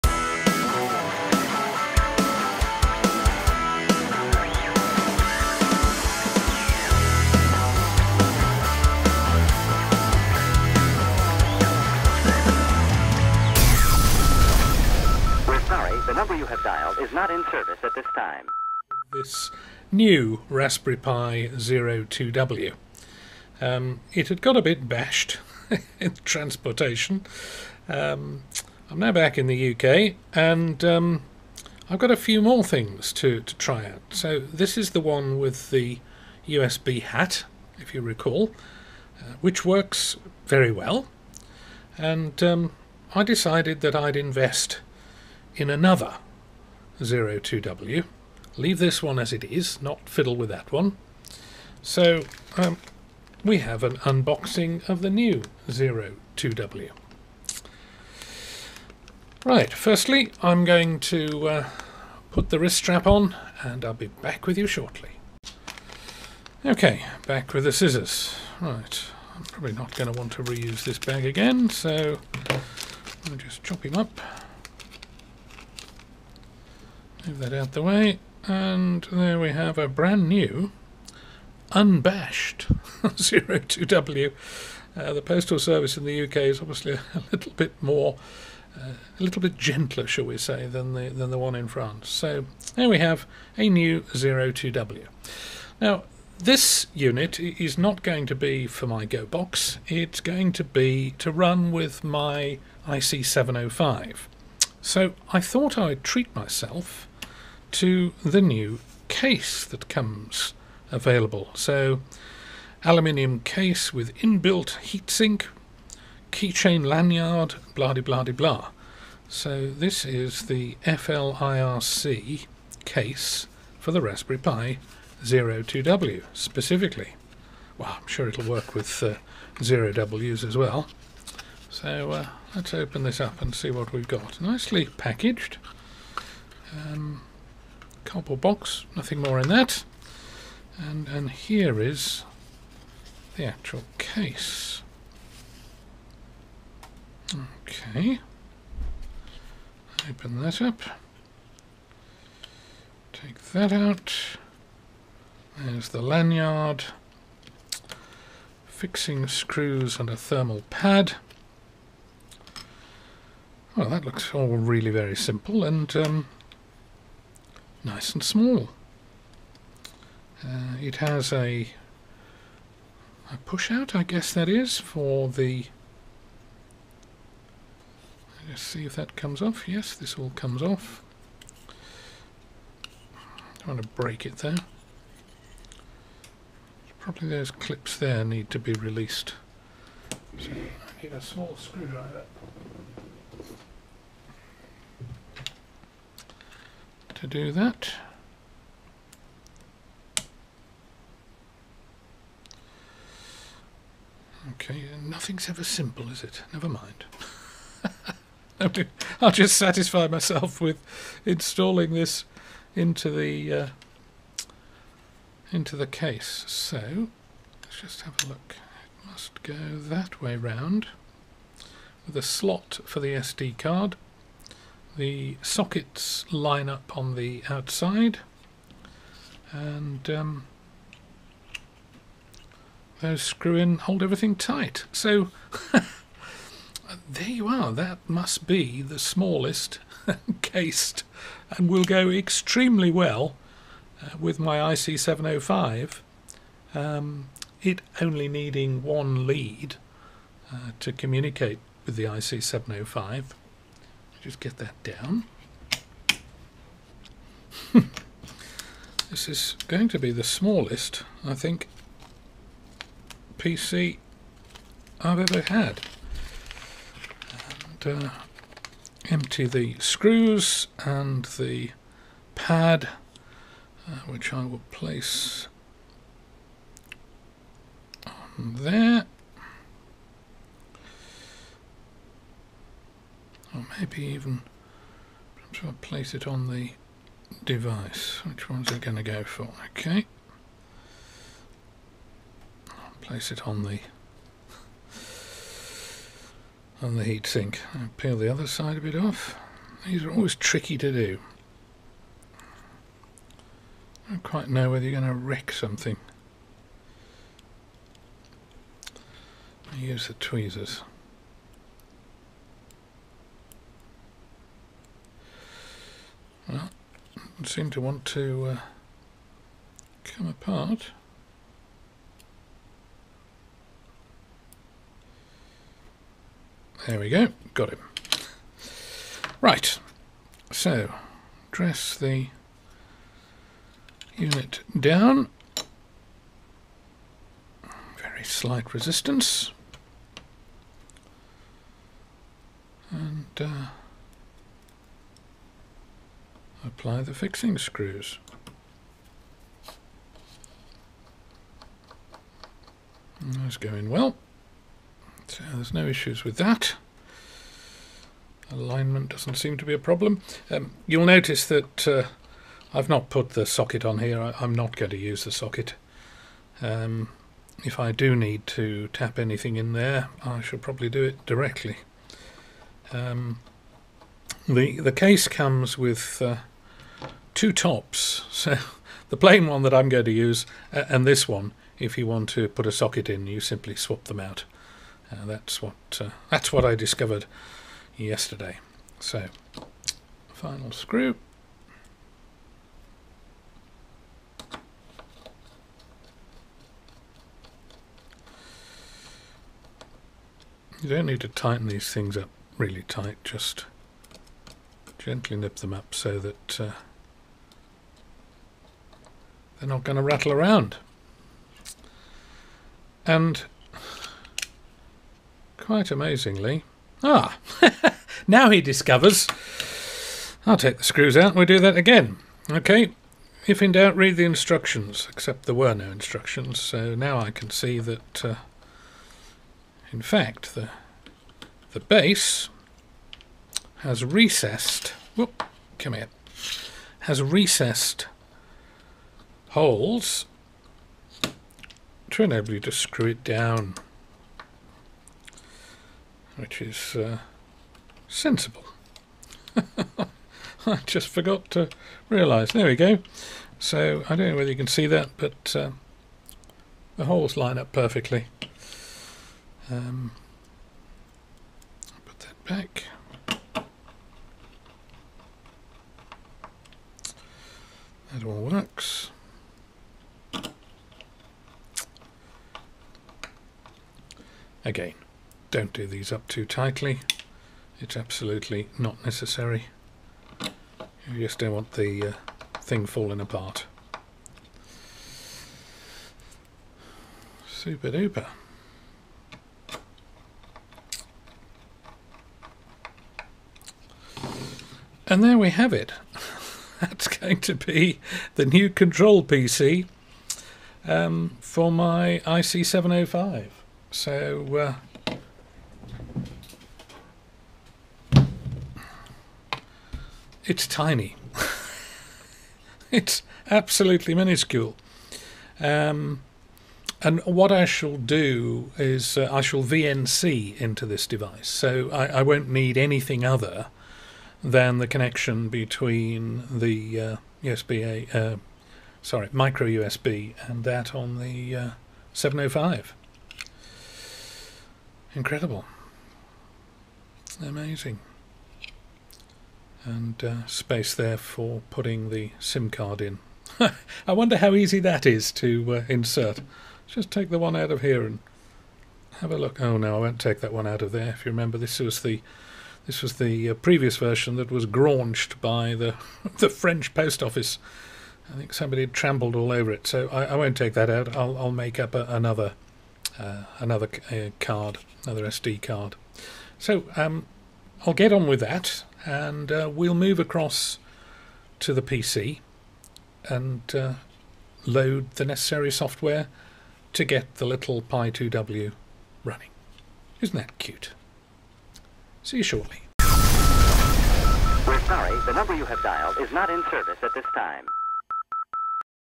Raspberry Pi zero 2 W for Ham Radio IC 705 part 1 hardware
This video shows the beginning of a new Raspberry Pi zero 2W build for my IC-705 (or IC-7300 or any rig with USB serial connection and internal sound card)

There are some interesting heat-dissipating aluminium cases being offered for the Pi Zero range.

Being so small and physically robust, This one is perfect for IC-705 portable operation -- and would be equally at home in your shack.

As always, there will be information on my web site to support this new video playlist.

Part 2 will explain about loading digital modes software and how to overclock the zero 2 w hardware.

Part 2 will be released in a few days, Part 3 will follow.

Have a great Christmas.

73
Bruce G4ABX
Web: g4abx.co.uk